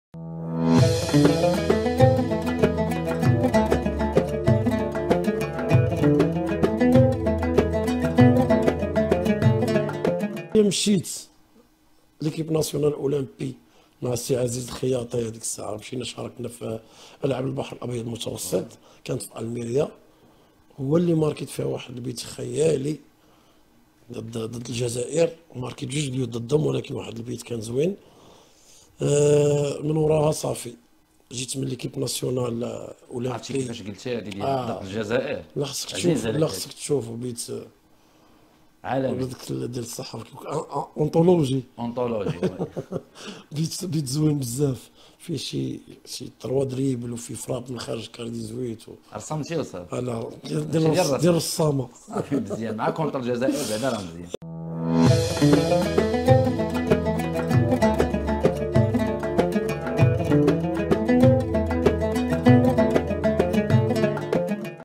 الرجاوي الصواري:رحيمي عندو تقنية خطيرة وكيعرف فين جات الشبكة..
كشف لاعب الرجاء السابق حسن الصواري لشوف تيفي تفاصيل وأسرار مثيرة.